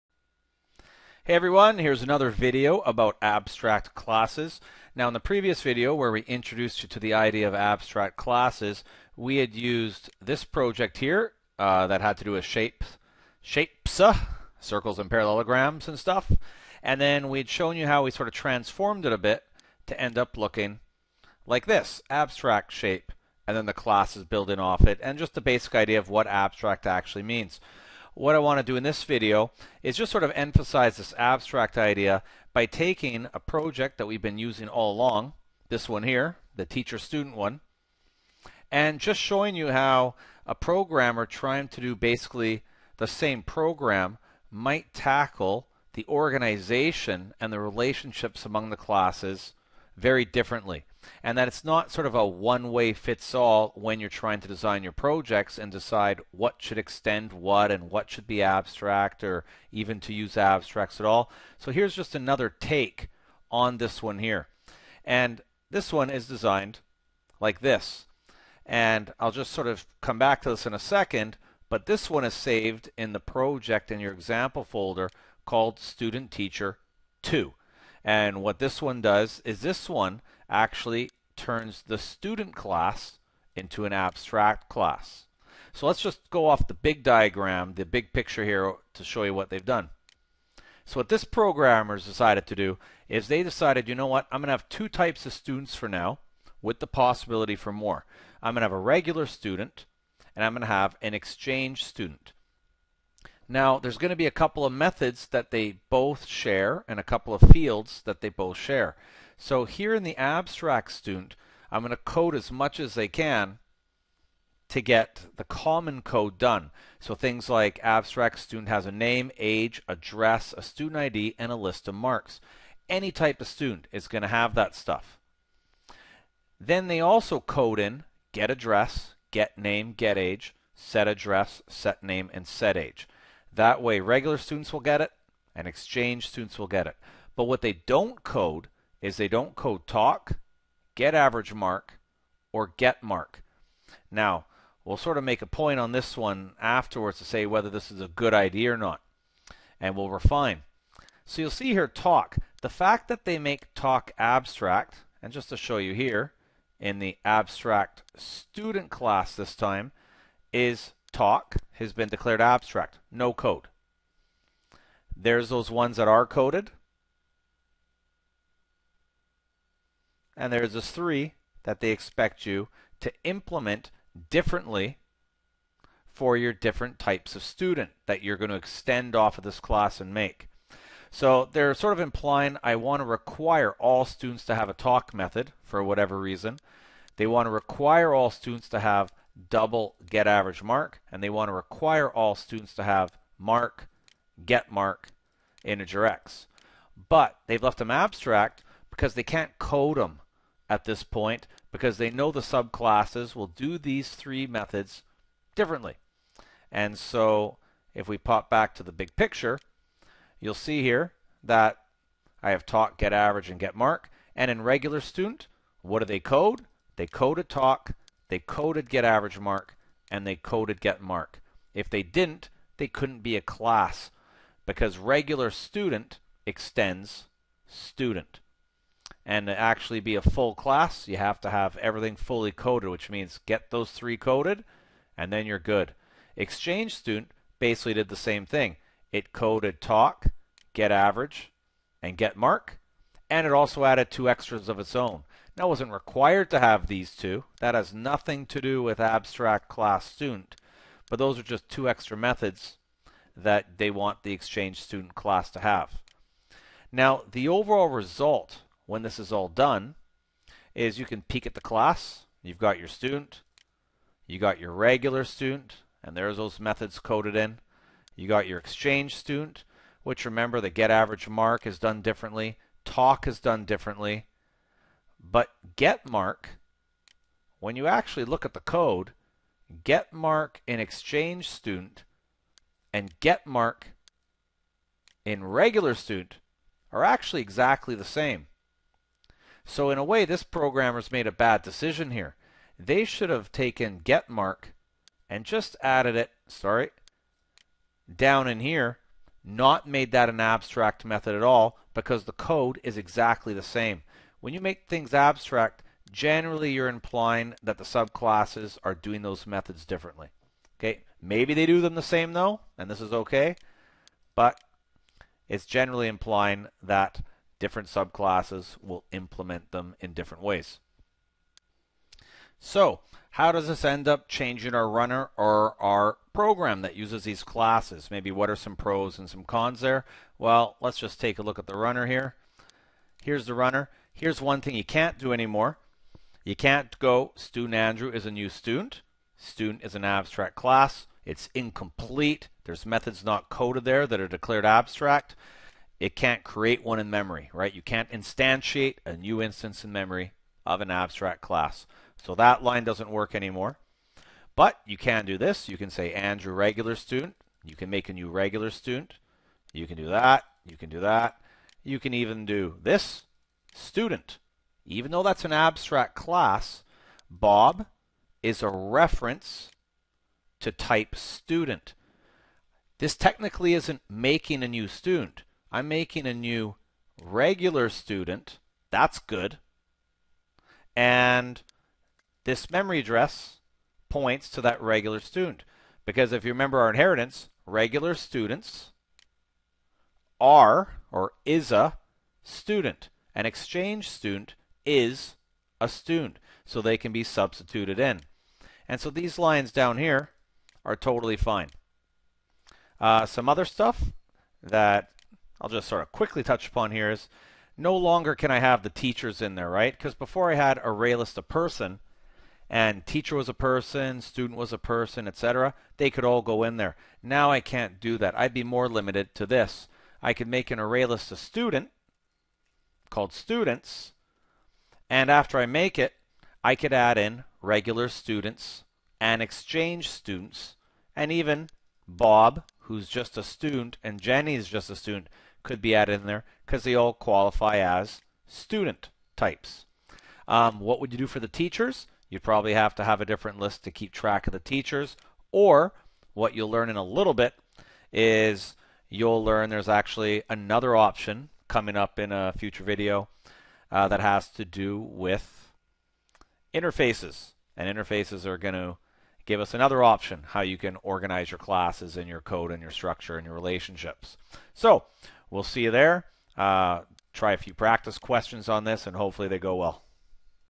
AP Computer Science 201 Abstract Classes 02
AP Computer Science Java tutorials and lessons that teach you how to program and code using Java.

has lessons and activities and resources that match these video tutorials and lessons for learning Java.
What is an abstract class in Java?  How to write an abstract class and why and when to use an abstract class.